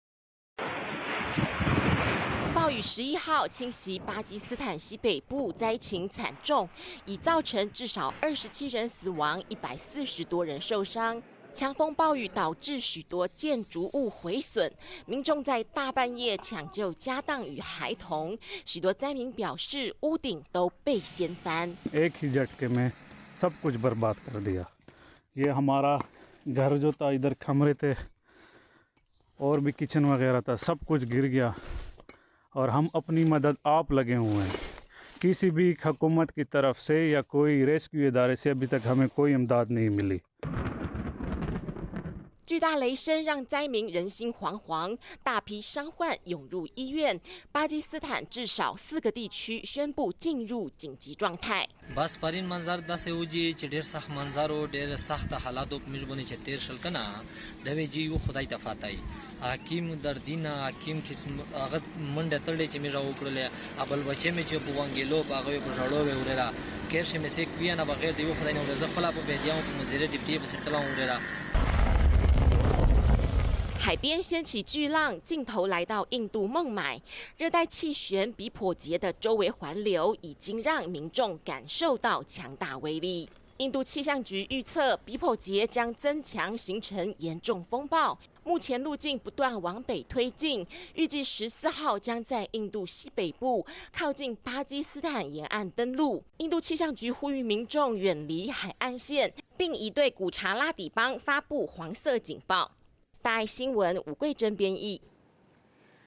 壞天氣襲擊巴基斯坦 熱帶氣旋又步步逼近
熱帶氣旋比頗傑一路往北推進，接近印度和巴基斯坦，預計14號在印度西北部、靠近巴基斯坦沿岸登陸。印度氣象局表示，比頗傑可能演變為極其嚴重的氣旋風暴，呼籲民眾遠離海岸線。印度也針對古茶拉底邦、薩拉沙脫卡奇海岸發布黃色警報。

暴雨11號襲擊巴基斯坦西北部，災情慘重，已造成至少27人死亡、140多人受傷。強風暴雨，導致許多建築物毀損。民眾在大半夜，搶救家當與孩童。許多災民表示，屋頂都被掀翻。

災民 夏哈：「暴風雨很強勁，一下子就摧毀了所有東西，我身旁的臥室跟廚房，都被吹垮了，沒有人來幫忙，政府搜救部門完全都沒派人來。」

巨大雷聲，讓災民人心惶惶。大批傷患湧入醫院，巴基斯坦至少四個地區，宣布進入緊急狀態。

災民 瑞曼：「暴風雨來的時候真的很恐怖，我這輩子沒看過這麼強的暴雨，大家都憂心忡忡，一整晚都不敢闔眼，暴風雨把所有東西都吹到天上，我們慌張到不知道，應該怎麼辦才好。」

海邊掀起巨浪，鏡頭來到印度孟買。熱帶氣旋比頗傑的周圍環流，已經讓民眾感受到強大威力。印度氣象局預測，比頗傑 將增強 形成嚴重風暴，目前路徑 不斷往北推進，預計14號將在印度西北部、靠近巴基斯坦沿岸登陸。

印度氣象局呼籲民眾，遠離海岸線。並已對古茶拉底邦發布黃色警報。

APTN：武桂甄 編譯 謝啟泉 剪輯